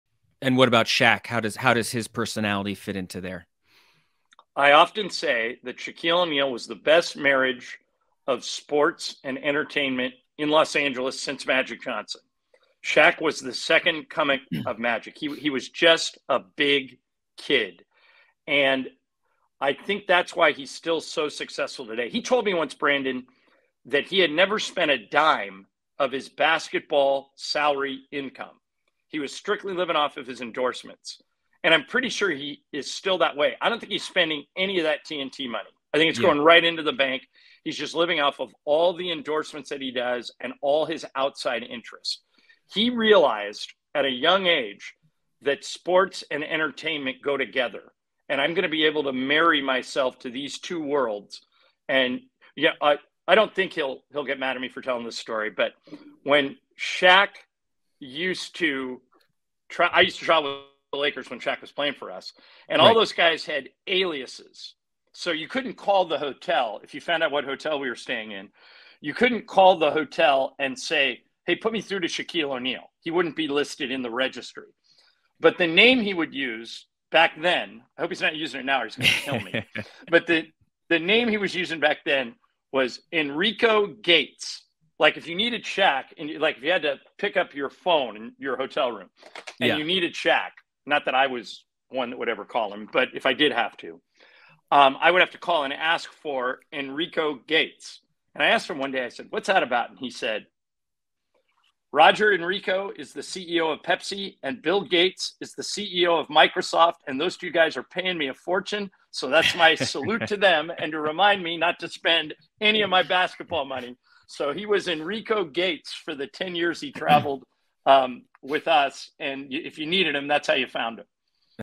Shaquille O'Neal didn't spend NBA money while playing, lived off endorsements
LA Lakers radio voice John Ireland revealed Shaquille O'Neal's spending habits, as well as the origin of Shaq's travel alias while playing for the Lakers.